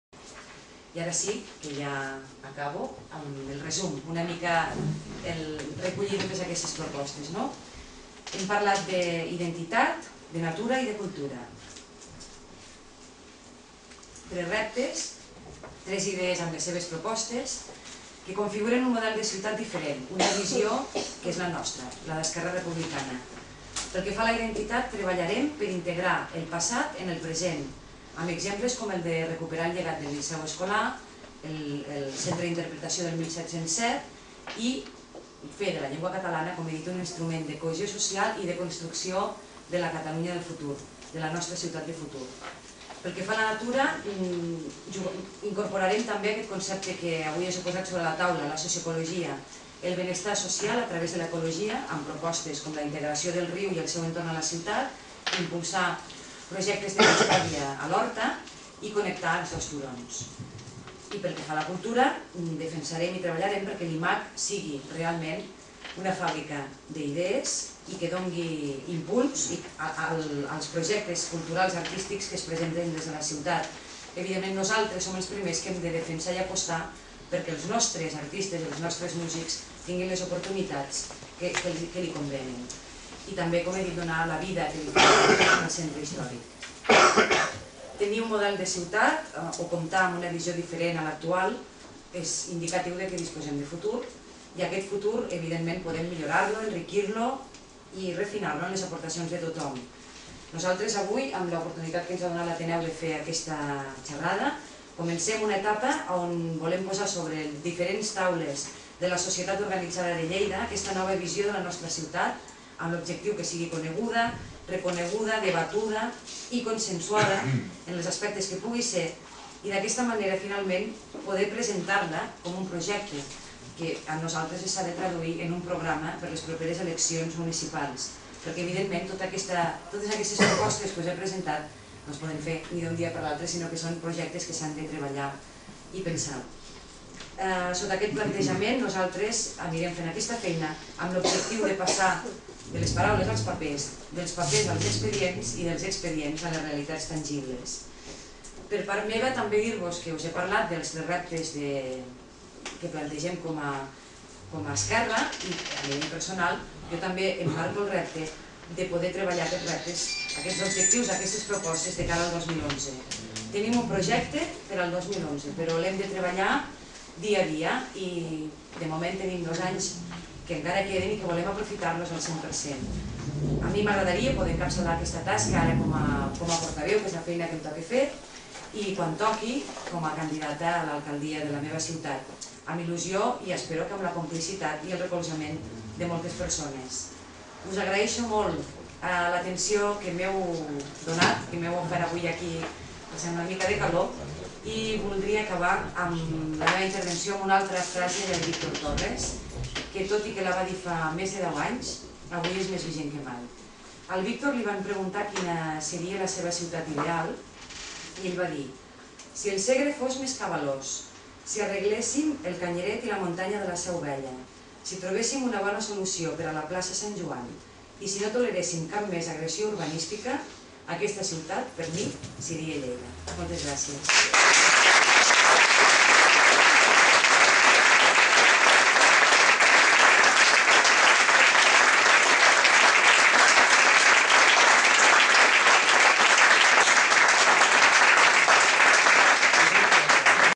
Lleida, una mirada endins (5/5)
Conferència de Montse Bergés a l'Ateneu Popular de Ponent